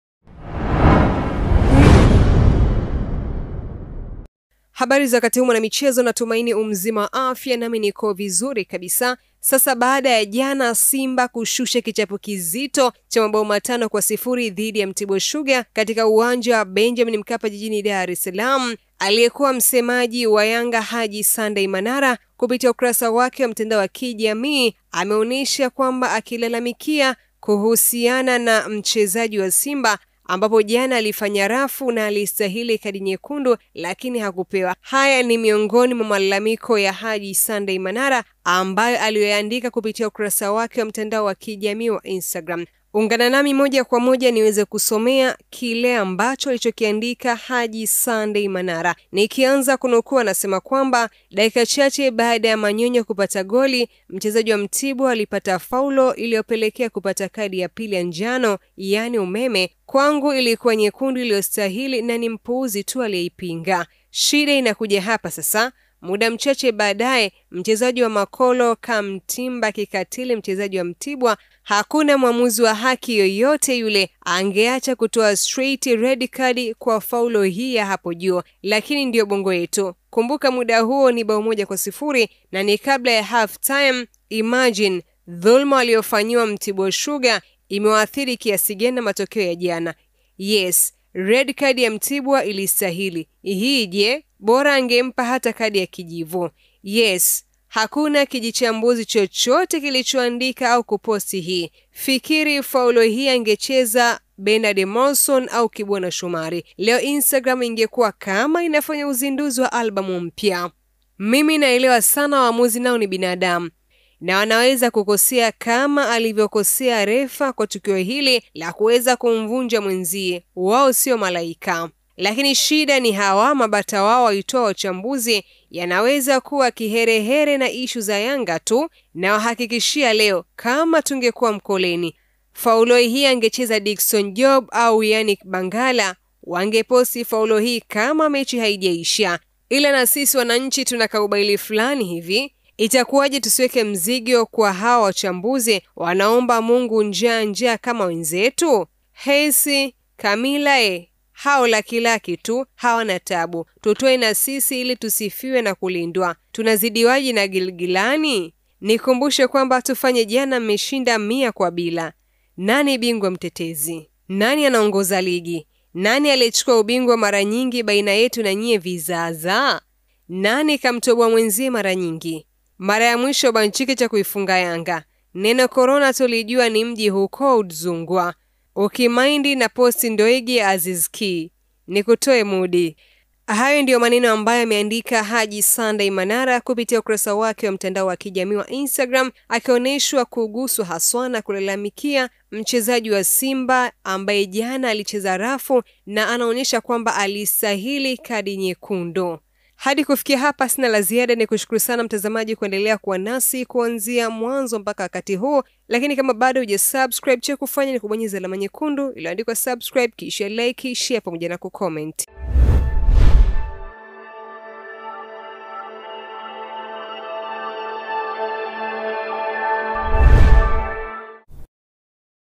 KIMENUKA! MANARA AMELIAMSHA DUDE HUKO! UMEONA? ATOA MALALAMIKO MAZITO LEO! WANAHONGA? AITAJA SIMBA
WAKIJIANDAA KUWAVAA WAARABU JUMATANO, UMEONA ALICHOSEMA MANARA LEO? ACHARUKA! ATOA TAMKO ZITO ETI NINI? AITAJA SIMBA WANAHONGA? KIMEUMANA CHEKI VIDEO NZIMA HII HAPA